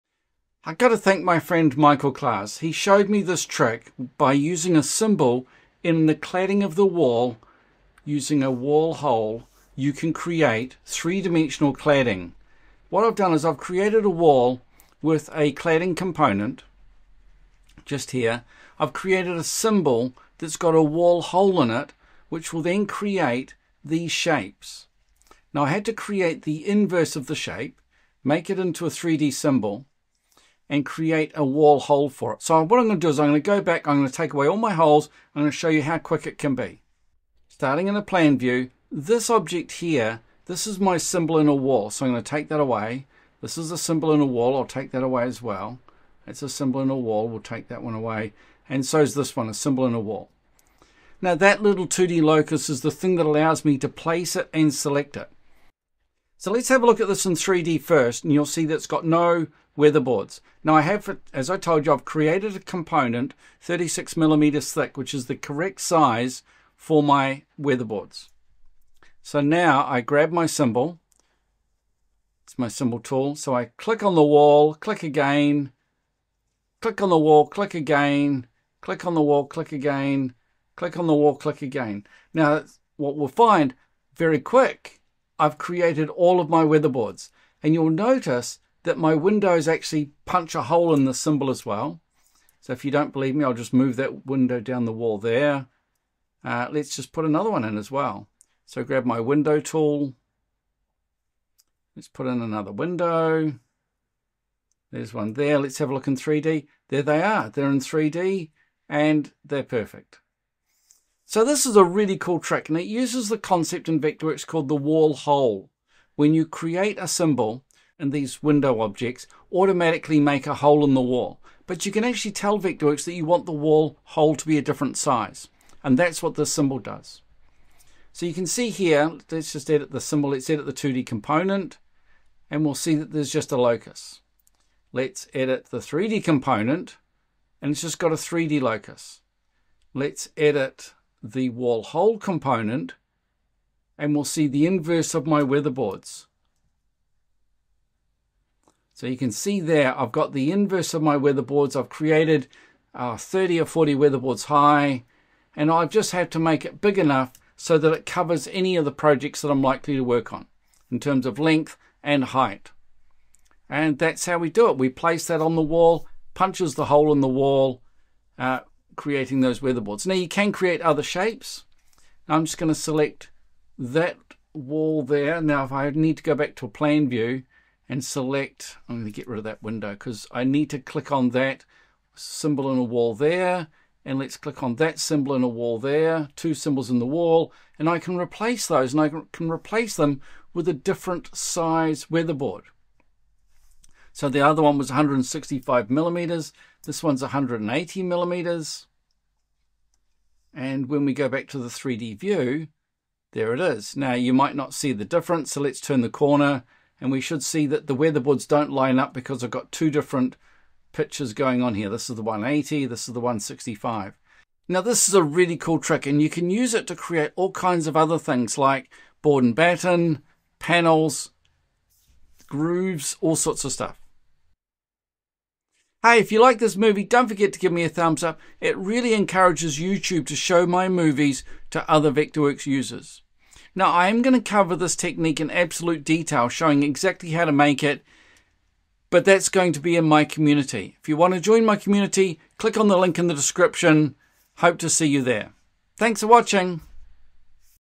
Create 3D Cladding in Vectorworks Using Wall Hole Symbols
In this episode, Jonathan unveils an ingenious method for creating three-dimensional cladding using Vectorworks. He shares a technique introduced by his colleague, Michael Claas, which involves utilizing symbols and wall holes to enhance architectural designs. Jonathan meticulously demonstrates each step of the process, highlighting its efficiency and effectiveness in transforming flat surfaces into dynamic 3D designs.

This episode delves deep into the intricacies of using Vectorworks to create custom weatherboards. Jonathan explains his process from concept to implementation, offering invaluable insights into creating wall-hole symbols. He guides listeners through the process of adjusting wall components and integrating windows with weatherboards, showcasing the seamless functionality of these techniques. This detailed tutorial not only emphasizes the customization potential within Vectorworks but also offers practical tips for design professionals seeking to enhance their digital architecture capabilities.

Key Takeaways:

*   **Vectorworks Symbols**: Learn how to use symbols and wall holes to create complex 3D cladding designs efficiently.
*   **Weatherboards**: Discover Jonathan's method for designing and implementing weatherboards in Vectorworks, focusing on precision and versatility.
*   **Integration Techniques**: Understand the process of integrating windows and other architectural features seamlessly with custom wall designs.
*   **Customization and Application**: Explore the potential for customization within Vectorworks, including various board sizes and design shapes.
*   **Community Learning**: Jonathan invites listeners to join his community for deeper insights and detailed tutorials on advanced Vectorworks techniques.